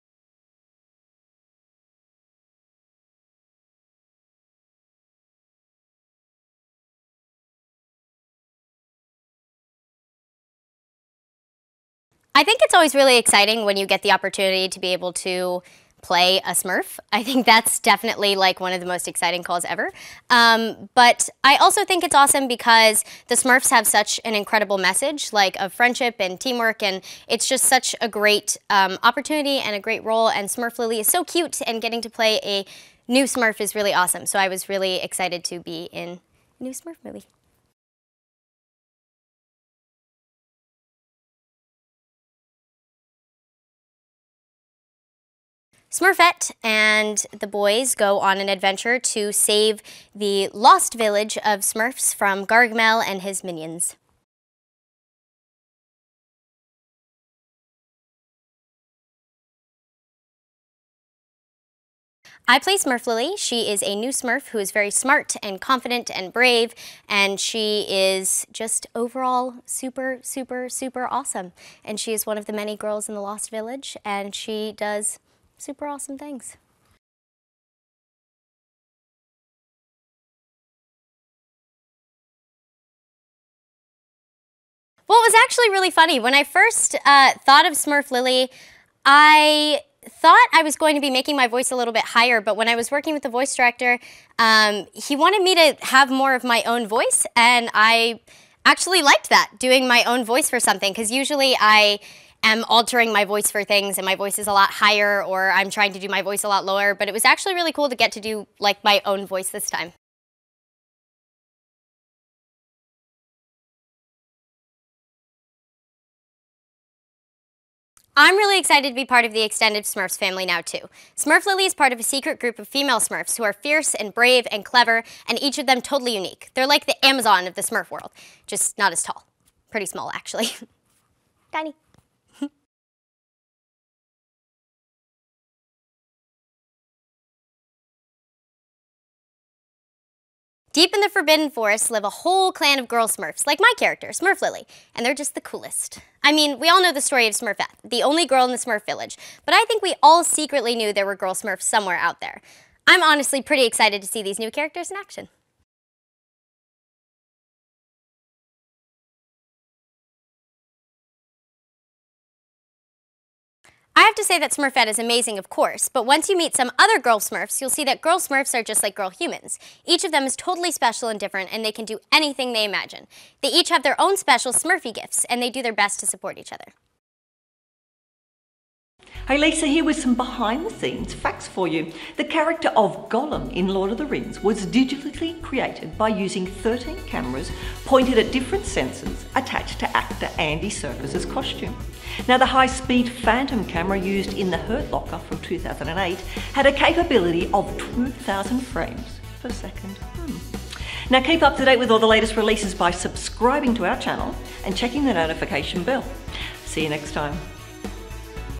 Smurfs: The Lost Village | On-set visit with Ariel Winter 'Smurflily'
Smurfs: The Lost Village - Ariel Winter 'Smurflily' [On-Set Interview]

In this fully animated, all-new take on the Smurfs, a mysterious map sets Smurfette and her friends Brainy, Clumsy and Hefty on an exciting race through the Forbidden Forest leading to the discovery of the biggest secret in Smurf history.